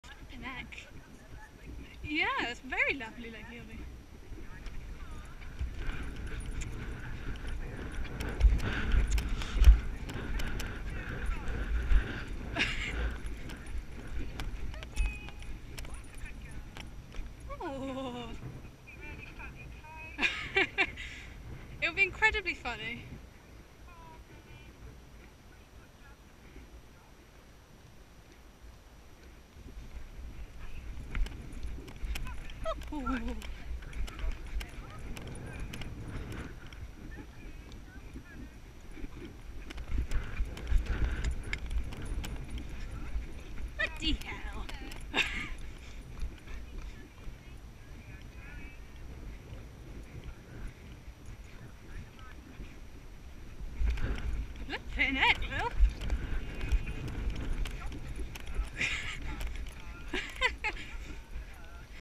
it's all unbearably exciting!
Just a few clips of my lesson the other day - Lily hadn't been jumping in the school for a while and she was a VERY keen bean!

She wasn't like this the whole time btw - she was absolutely fine for most of the lesson, but she just couldn't contain herself on a couple of occasions!

Horse/Pony: Lily, 13.2HH Liver Chestnut Mare
Camera: GoPro Hero 3+